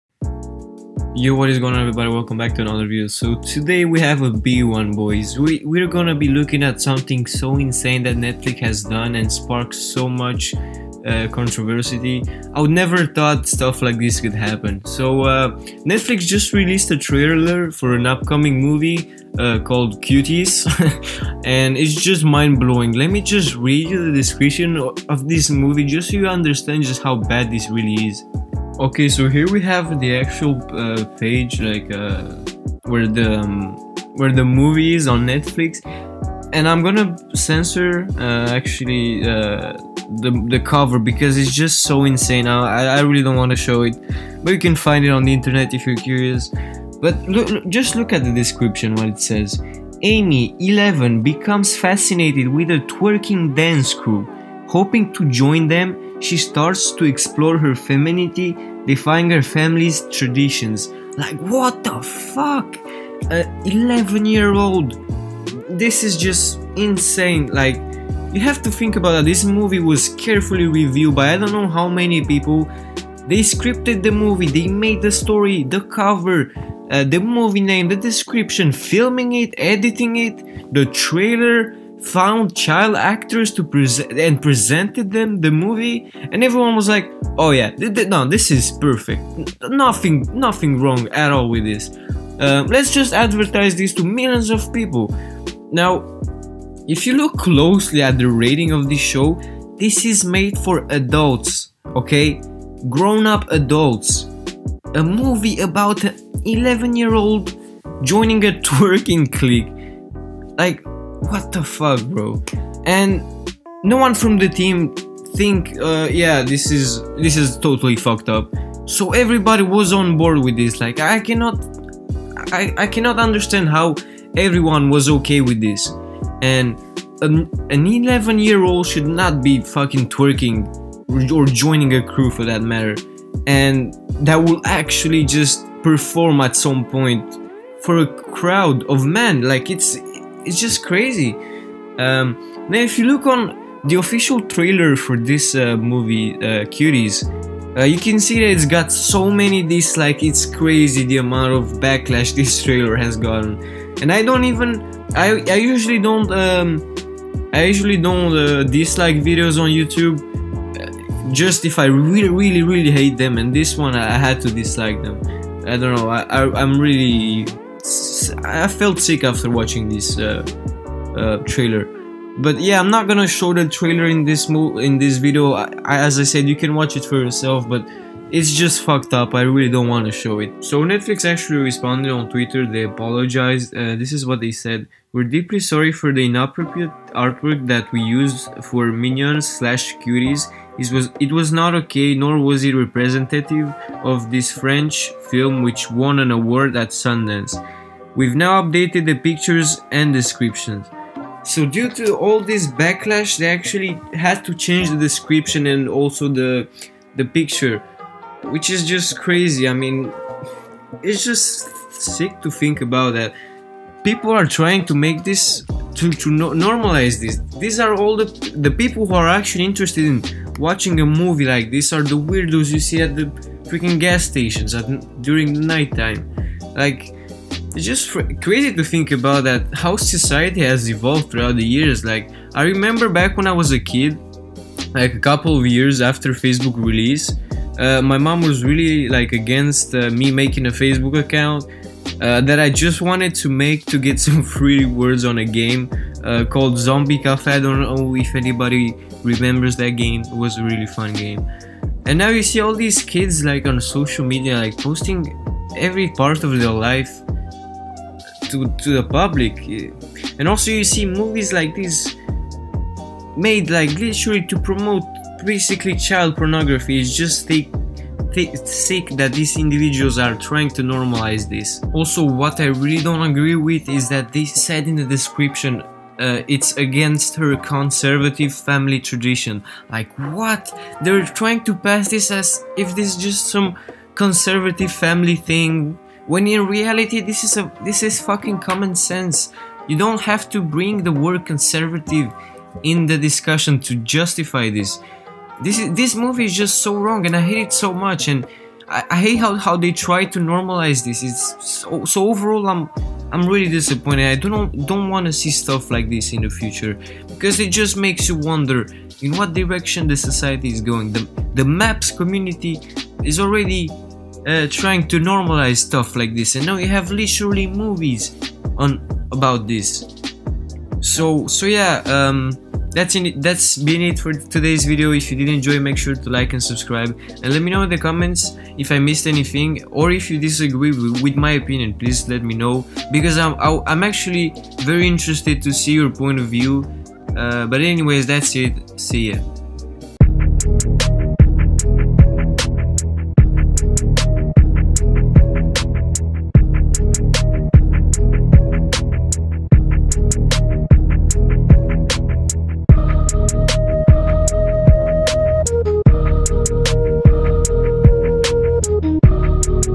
Netflix Is Cancelled... (Cuties)
This is my take on this whole situation regarding the drama for the movie called "Cuties" That literally feature under aged children and sexualizing them , I find it to be very wrong.

Just the name itself "Cuties" to be next to 11 year old is just messed up.